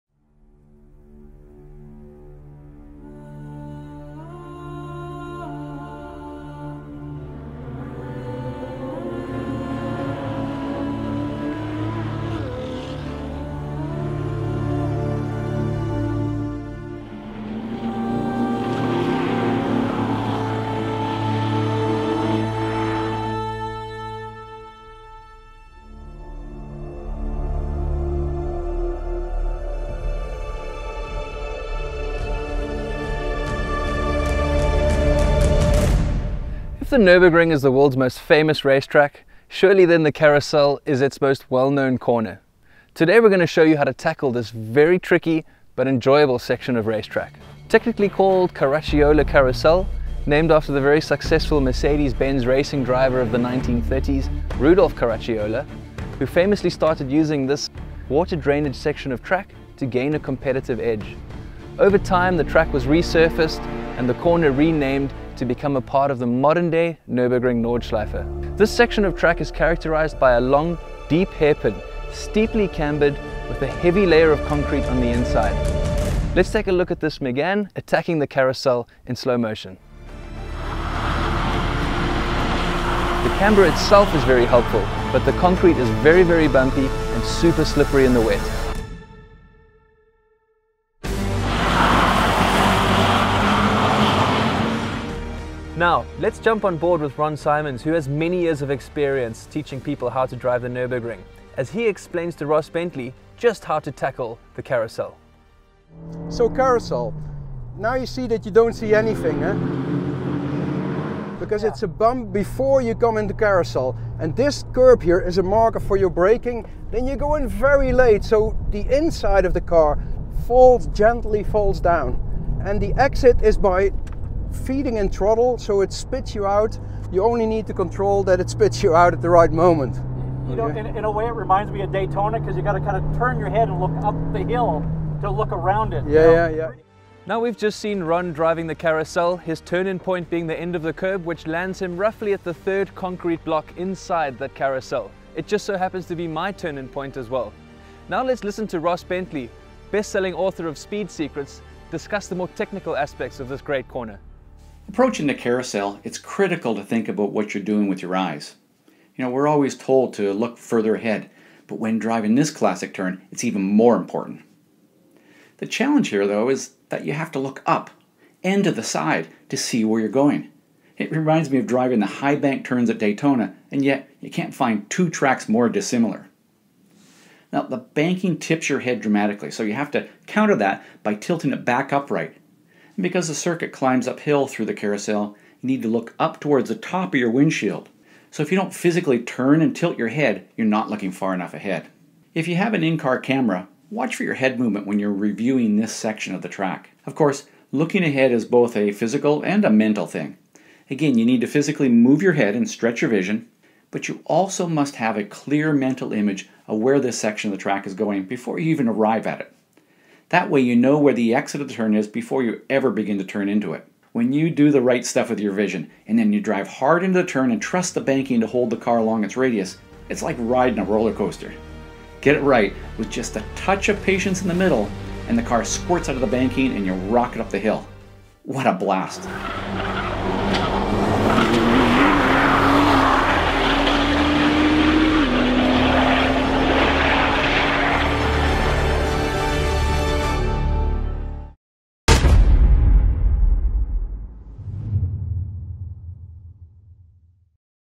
How to Drive Nurburgring's Most Famous Corner
If Nürburgring is world's most famous circuit then the Karussell is for sure it's most recognisable corner. In this video we explain how this corner got it's name but more importantly what is the correct way around it.
Ross Bentley is the author of the best-selling book series, Speed Secrets. Ron Simons has been an instructor with major manufacturers, like Ferrari and Porsche, and has been teaching people how to drive the 'Ring for the last 20 years.